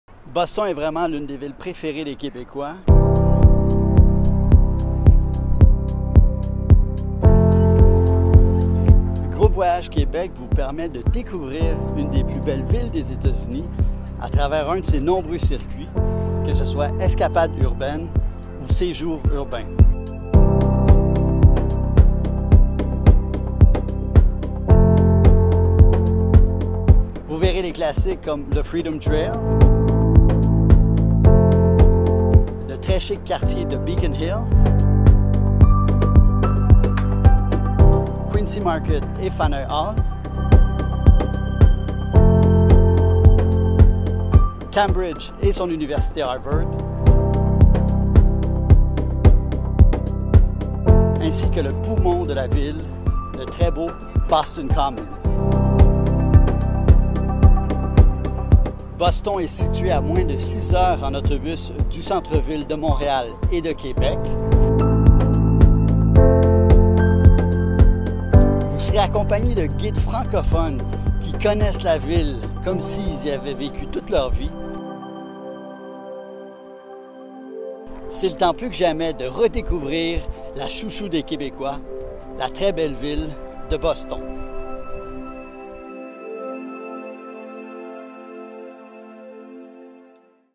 Escapade urbaine à Boston avec Jean-Michel Dufaux
Découvrez Boston et son histoire à travers son architecture variée juxtaposant gratte-ciel et vestiges coloniaux. Suivez la célèbre Freedom Trail et visitez des lieux mythiques tels que le quartier Beacon Hill, le Quincy Market, l’Université Harvard, le Fenway Park ou profitez d’un moment de détente dans le Boston Common, le plus ancien jardin public des États-Unis. Une ville aux multiples possibilités pour satisfaire toutes vos envies!